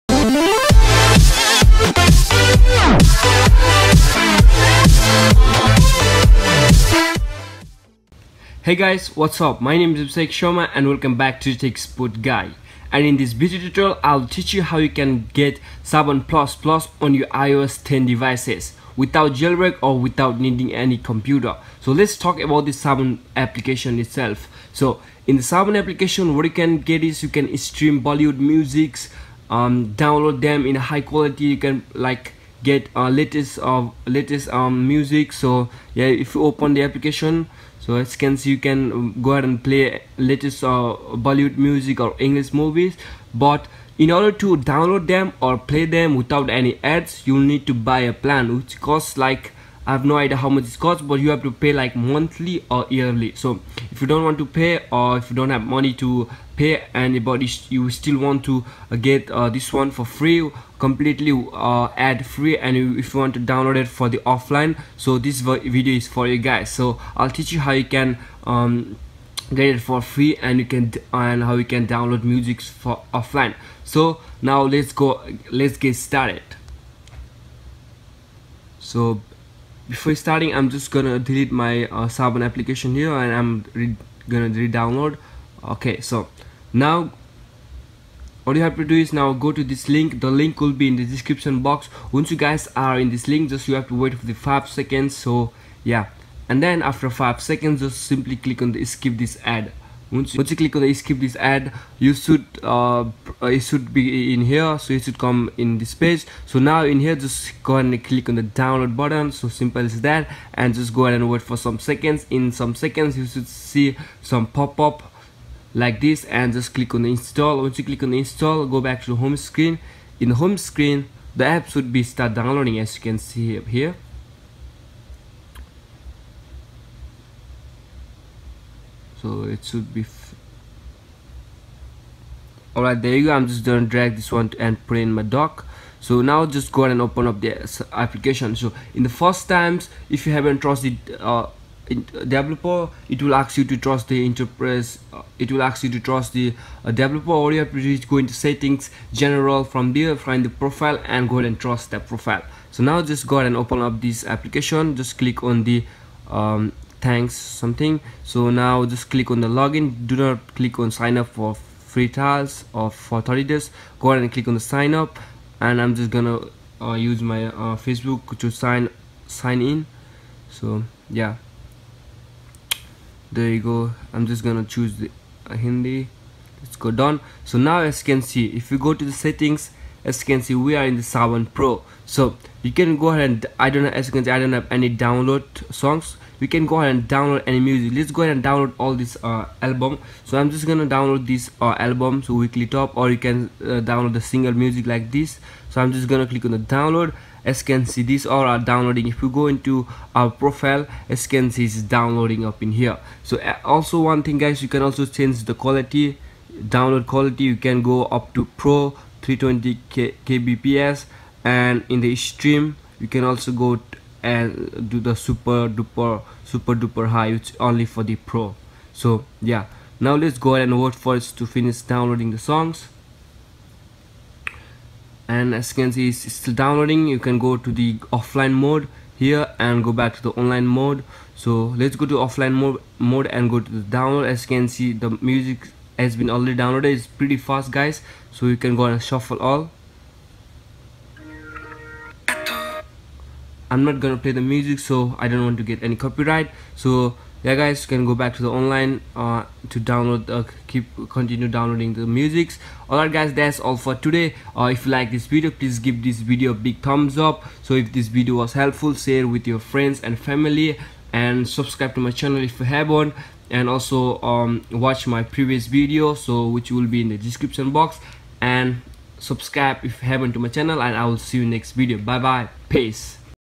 How to get Saavn++ pro on iPhone, iPad, iPod Touch (NO JAILBREAK) (NO Computer) iOS 10/ iOS 9
How to get Saavn++ pro on iPhone, iPad, iPod Touch (NO JAILBREAK) (NO Computer) iOS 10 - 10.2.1 / iOS 9
Saavn Hack

Hey guys I'm close to 1000 subscribers, please help me to reach 1k subscribers.

Others Video on my channel